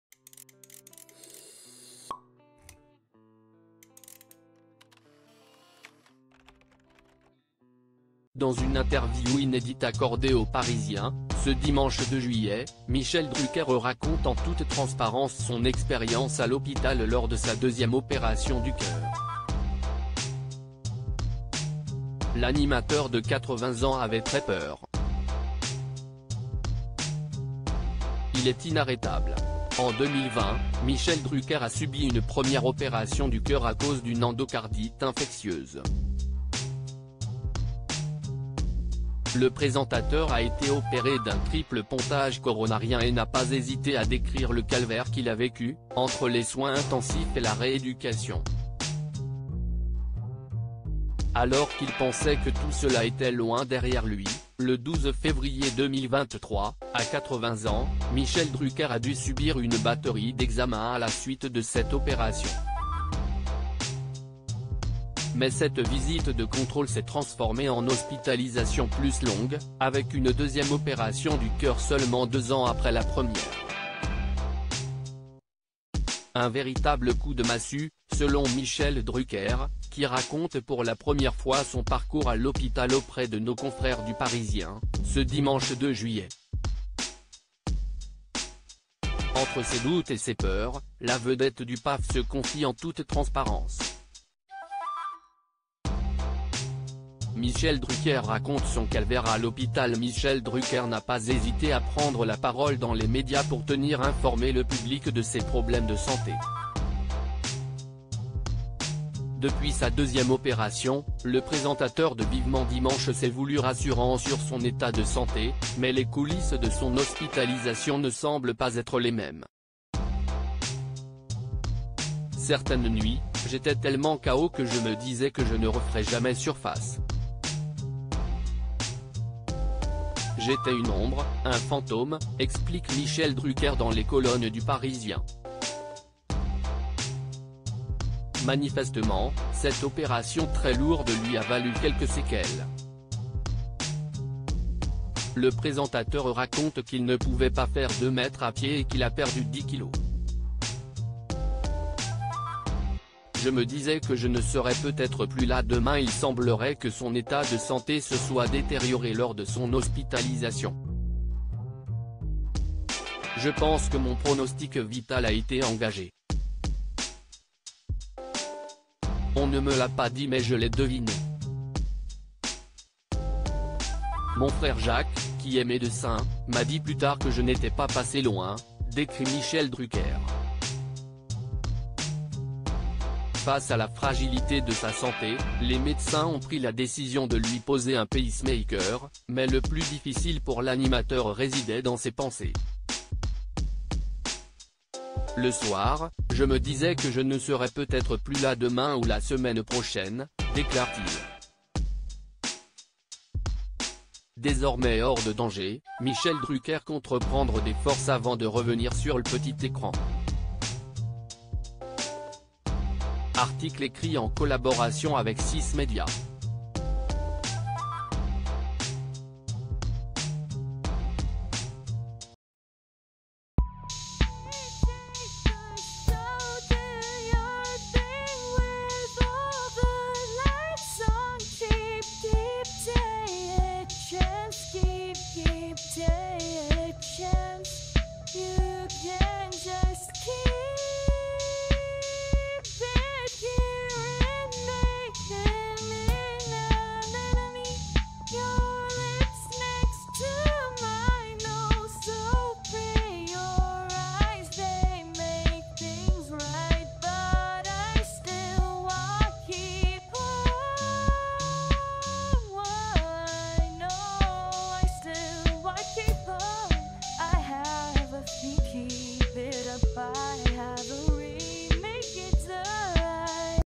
"Mon pronostic vital a été engagé" : Michel Drucker à nouveau opéré du cœur, il revient sur ces dern
"Mon pronostic vital a été engagé" : Michel Drucker à nouveau opéré du cœur, il revient sur ces derniers mois où il ne pensait pas s'en sortir